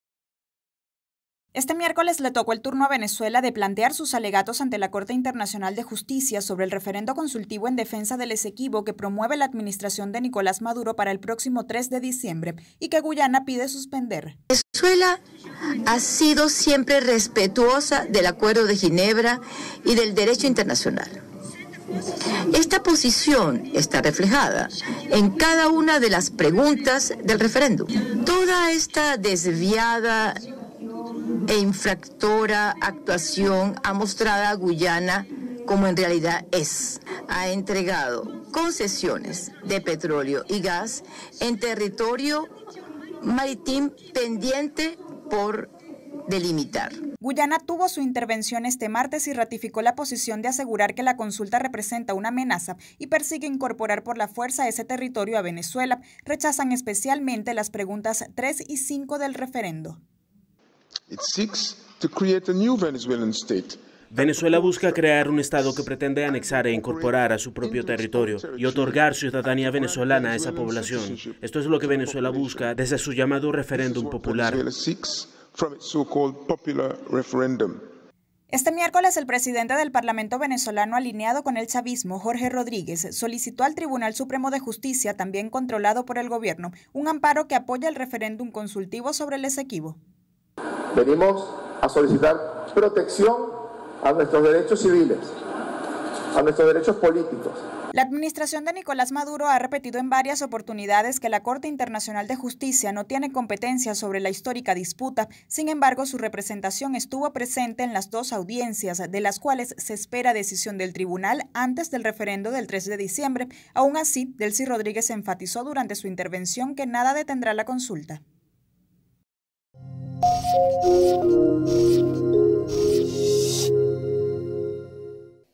Finalizaron audiencias ante la Corte Internacional de Justicia sobre el Esequibo - 15/11/23
El Gobierno de Guyana ratifica ante la Corte Internacional de Justicia solicitud de medidas contra el referéndum por el Esequibo y la administración de Nicolás Maduro rechaza estas estas acciones en La Haya.

Guyana indica que solo la Corte Internacional de Justicia tiene el poder jurídico de proteger el Esequibo, la defensa de Guyana calificó las preguntas del referéndum como lejos de ser neutrales, mientras que el Gobierno guyanés señaló que luego del referéndum Venezuela podría utilizar a las Fuerzas Armadas para tomar por la fuerza el territorio en disputa.

Reporta @luiskparada

Conéctate al canal 175 de @vix y disfruta de esta y otras informaciones desde cualquier parte del mundo.